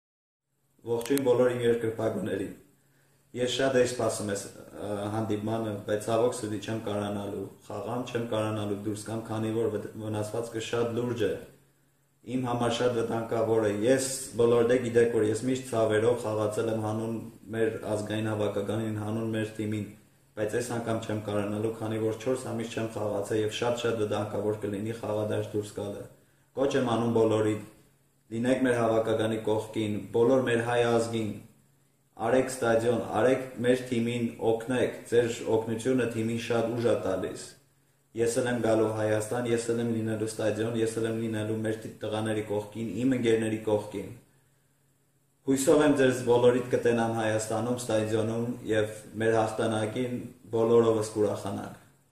Կոչ եմ անում բոլորին լինել մեր հավաքականի կողքին. Յուրա Մովսիսյան
Հայաստան-Սերբիա խաղից առաջ Յուրա Մովսիսյանը դիմել է երկրպագուներին